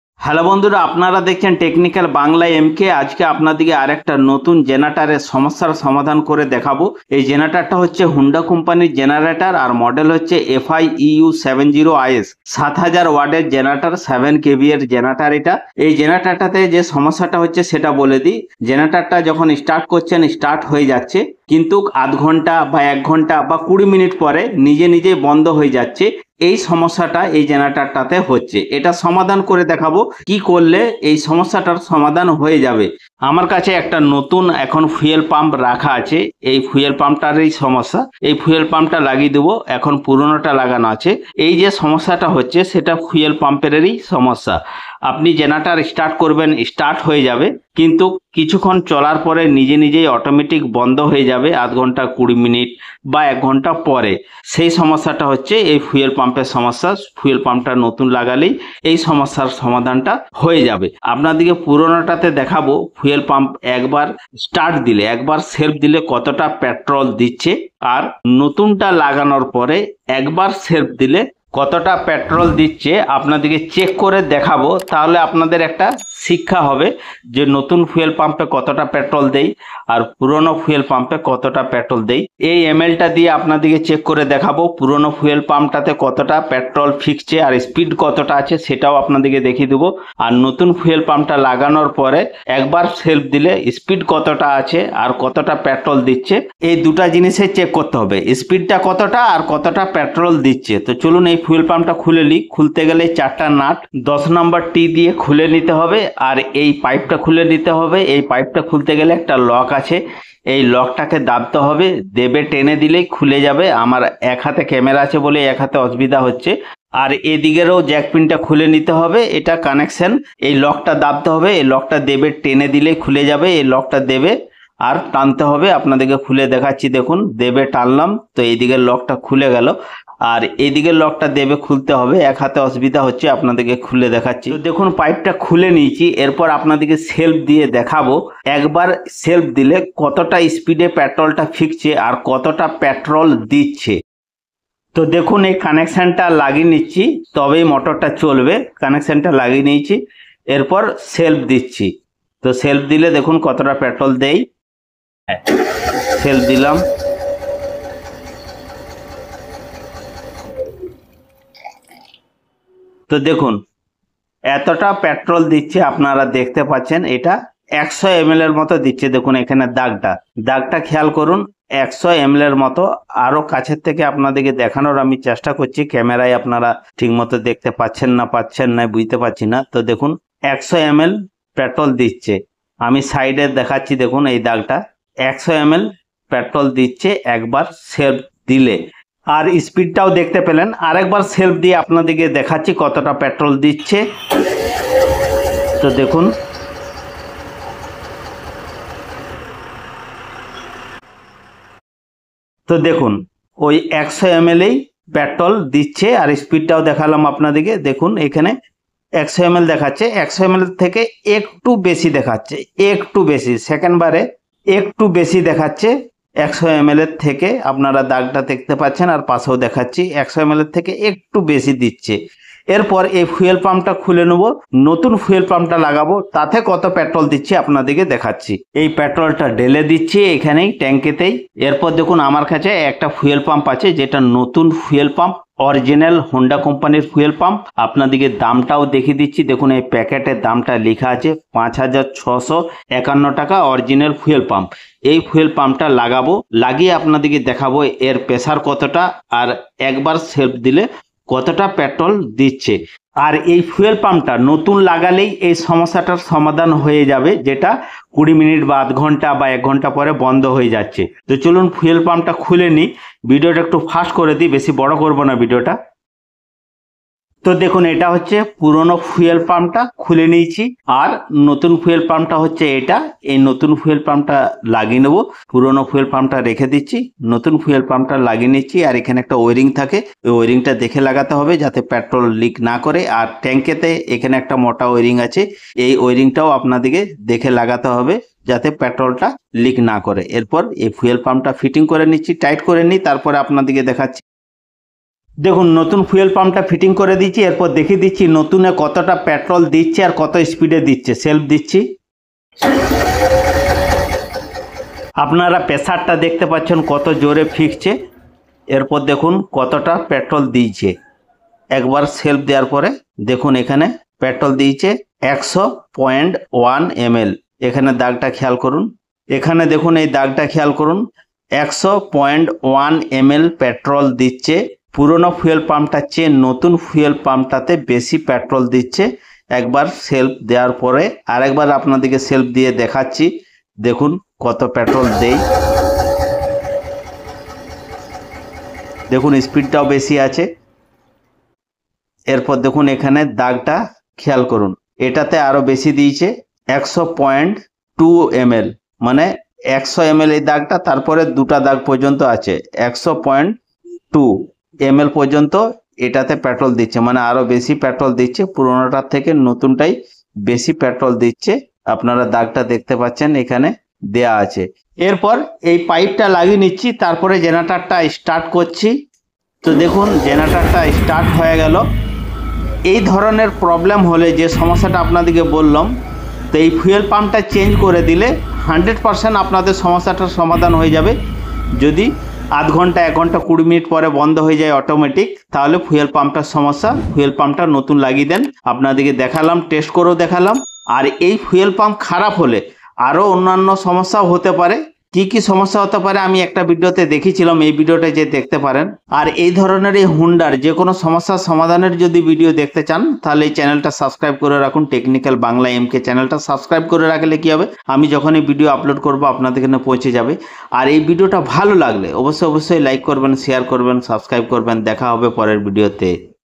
হুন্ডা জেনারেটর স্টার্ট হচ্ছে কিন্তু কিছুক্ষণ পর বন্ধ হয়ে যাচ্ছে কি করে ঠিক করব
Honda generator eu70is starting problem
হুন্ডা জেনেটার স্টার্ট হচ্ছে কিন্তু কিছুক্ষণ পরে বন্ধ হয়ে যাচ্ছে যা কি করে ঠিক করব

My tools
E bike testing device

6, 12v Dc Battery Meter

Electric Screwdriver